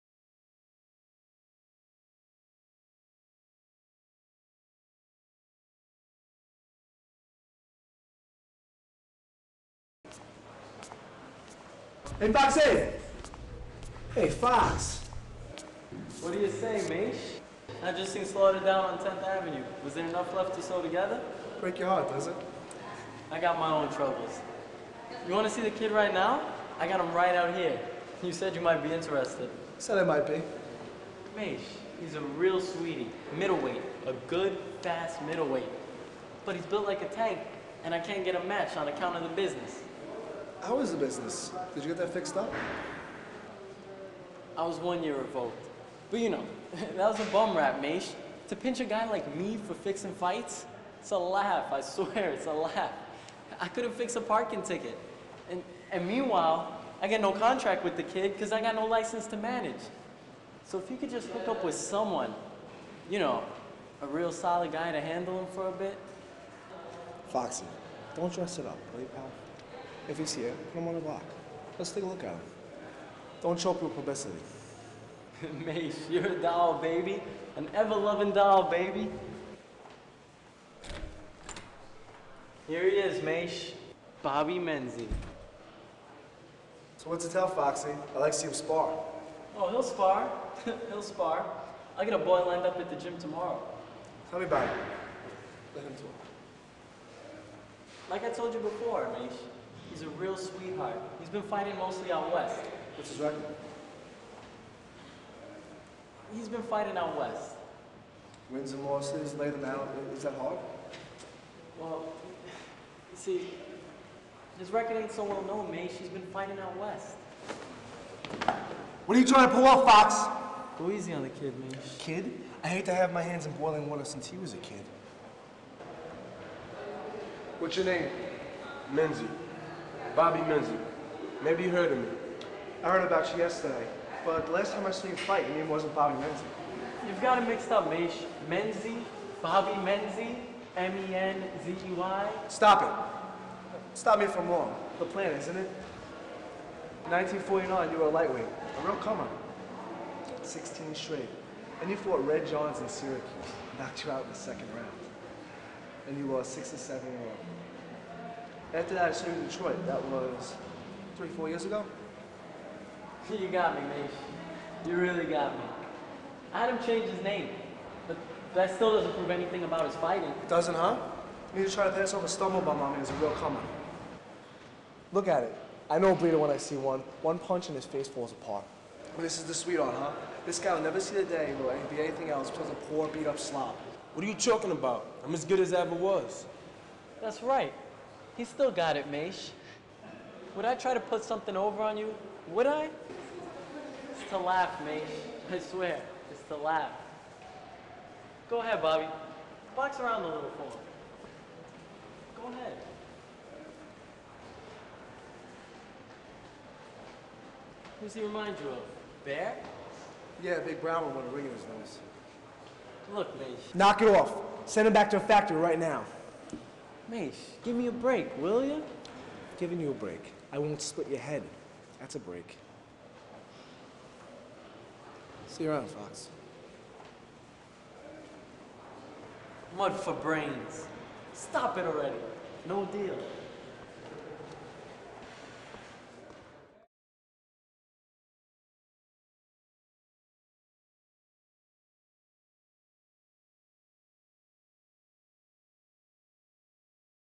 "Requiem for a Heavyweight" Scene
A scene from Rod Serling's "Requiem for a Heavyweight." It was shot at Carnesecca Arena at the St. John's University campus. The scene is directed by Reed Frazier. It stars:

Ralph Raymond as Maish
Kris Fernandez as Fox
Eugene Lawrence as The Fighter

Special thanks goes to David Lee, the boom operator, and Melissa Martinez, the camera operator.

St. John's University
Queens, New York
College of Professional Studies
Television & Film Department